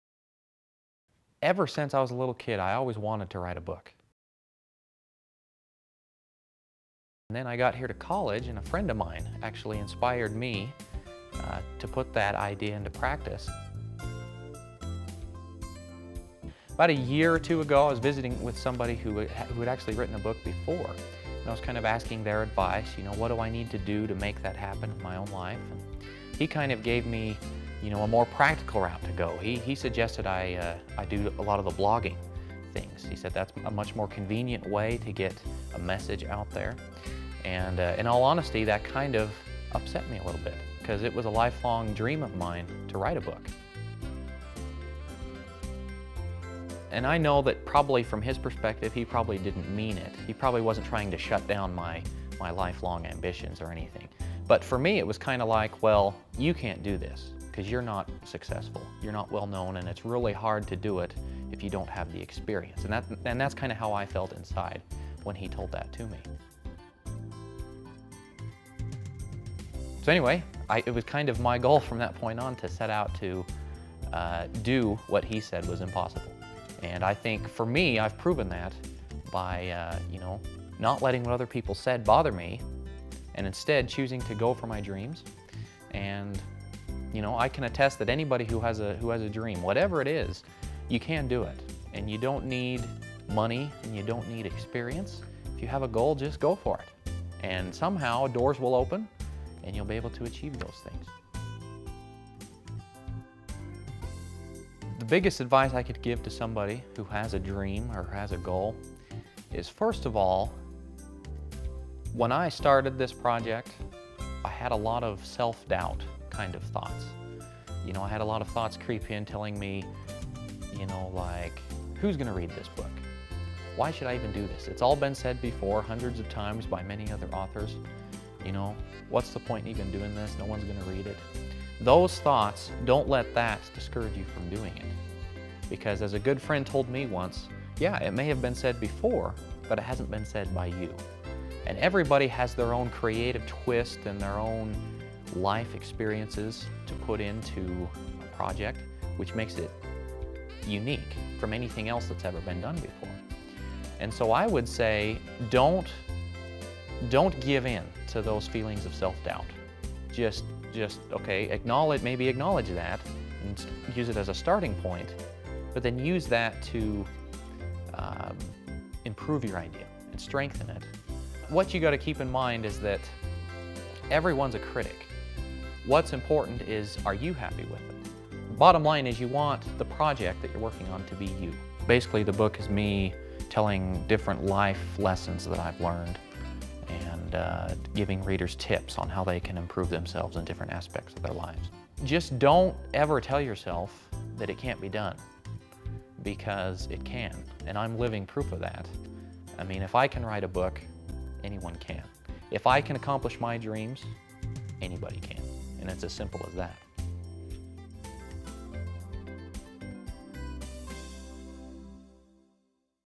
Rett Nelson Live.mp4
Rett Nelson talks about how anyone can achieve a dream if they just go for it and don't listen to critics.